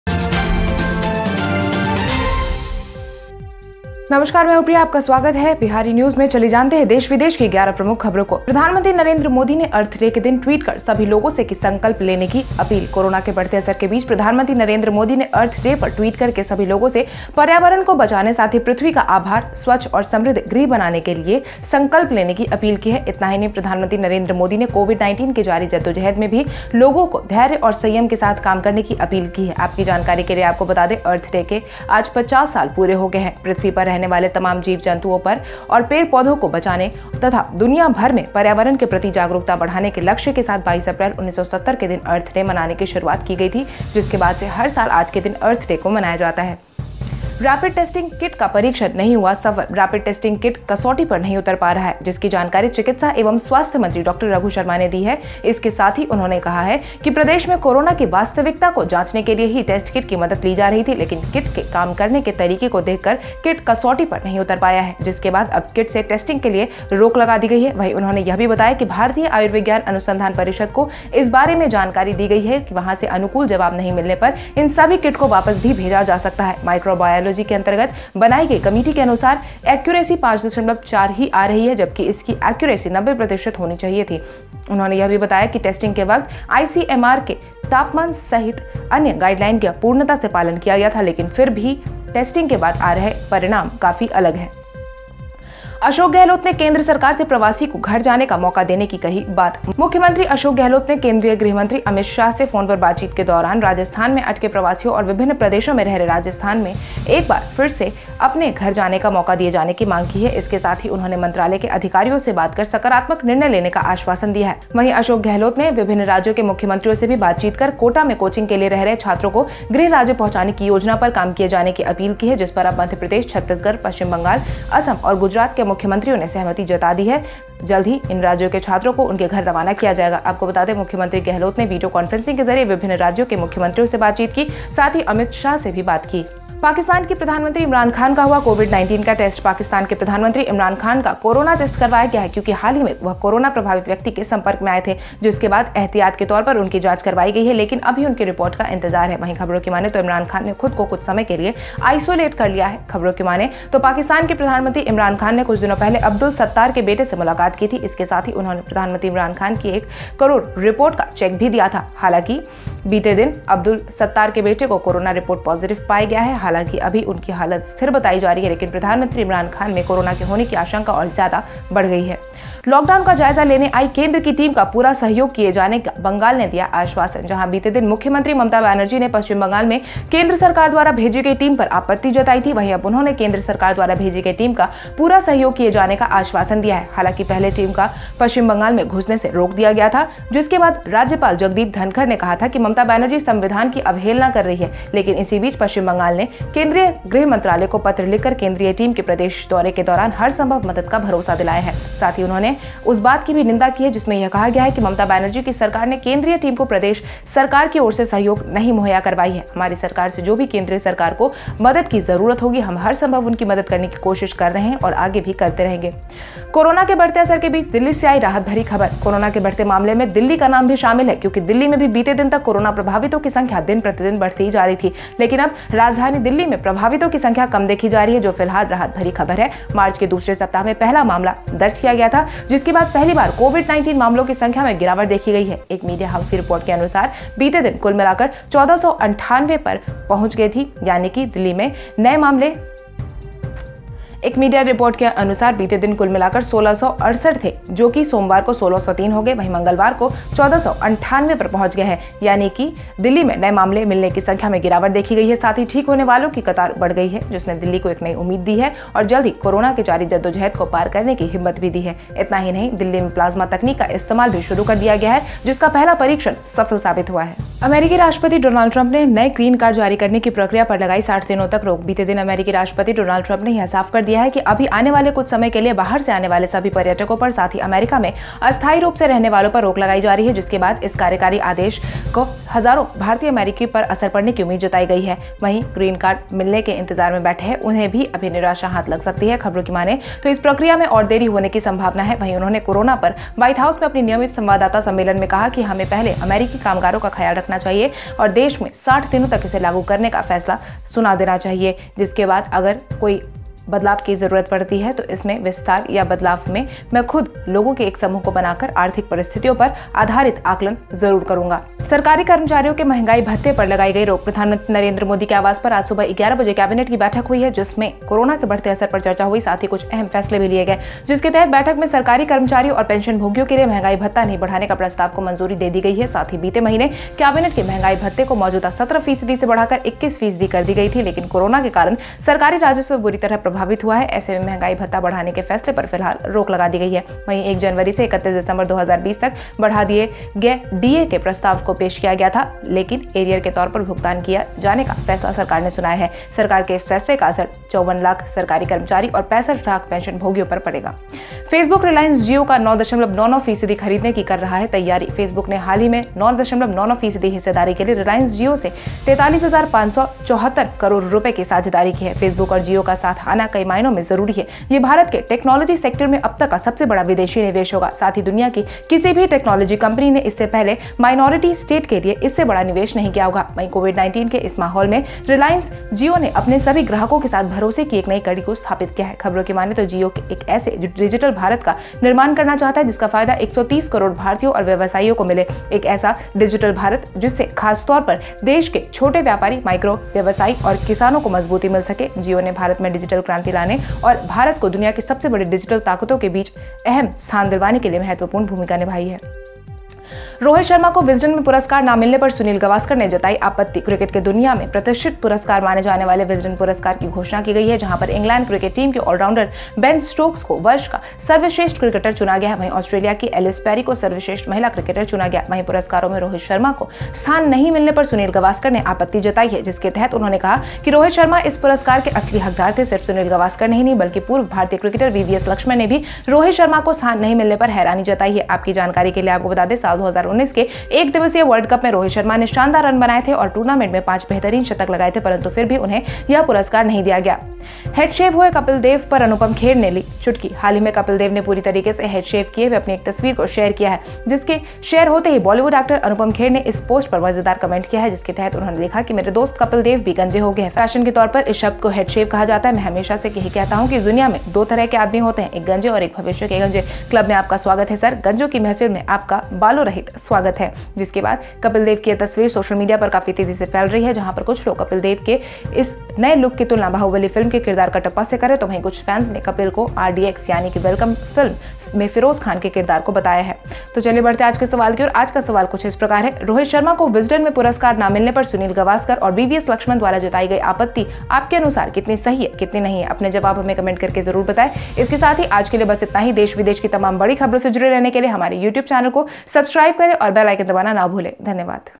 Get trending news of india and worlds.Info on, Covid 19 Test, Imran Khan, Donald Trump, Facebook,jio
Get latest today i.e. 22nd april news on Central Government, Dr. Raghu Sharma,IMCR, Ashok Gehlot, Covid 19 Test, Imran Khan, Donald Trump, Facebook,reliance jio, Sunil Gavaskar, Rohit Sharma, Anupam Kher.
1. PMnarendramodi to tweet on Earth Day and appeal to all people to take a pledge
2. Rapid testing kit not tested successfully
Rapid testing kit is not able to stand the test. Information about this has been given by DrRaghusharma, Medical and Health Minister. Along with this, he has said that the help of test kit was being taken to check the reality of corona in the state. But the kit has not been able to stand the test by giving way to how the kit works. After which the kit has now been banned for testing. At the same time, he also informed that information about this has been given to the Indian Council of Medical Research(IMCR), all these kits can also be sent back if there is no favorable response from there. According to the committee formed under microbiology, the accuracy is only 5.4%, while its accuracy should have been 90%. He also stated that at the time of testing, other guidelines including temperature of ICMR were followed thoroughly.
3. Ashokgehlot talks to central government to give migrant a chance to go home
4. Pakistan Prime Minister
5. Bengal assures full support of the team of Central Center to take stock of lockdown
6. Relieved news from Delhi amid Corona  growing impact
7. US President Donaldtrump imposes 60 days ban on the issuance of new green cards
8. There may be a ban on increasing dearness allowance of government employees
There is a cabinet meeting at 11:00 am today at the residence of Prime Minister Narendra Modi. In which the growing impact of Corona was discussed and some important decisions have also been taken. Under which the meeting has approved the proposal to not increase dearness allowance for government employees and pensioners. Partner: Last month, the cabinet had increased dearness allowance from the current 17 percent to 21 percent, but due to Corona, the government revenue has been badly affected, so the decision to increase dearness allowance has been put on hold for the time being. At the same time, a proposal not to be extended from January 1 to December 31, 2020 has also been introduced. Further payments will be made as arrears, this decision of the government will affect 54 lakh government employees and 65 lakh pensioners.
9. Facebook is preparing to buy 9.99% of Reliance Jio
Facebook recently partnered Rs 43574 crore from Reliance Jio for 9.99% stake. The coming together of Facebook and Jio is important in many ways. This is the largest foreign investment so far in India's technology sector. Also, no technology company in the world has made a big investment for a minority stake before. In this environment of Covid-19, Reliance Jio has established a new link of trust with all its customers. According to the news, Jio wants to build a digital India that will benefit 130 crore Indians and businesses. A digital India, especially to strengthen the small traders, micro traders and farmers of the country. Jio has played an important role in bringing digital revolution in India and making India a place among the world's largest digital forces.
10. Sunilgavaskar objected to Rohitsharma not receiving the award at Wisden
11. Anupamkher pinch heads over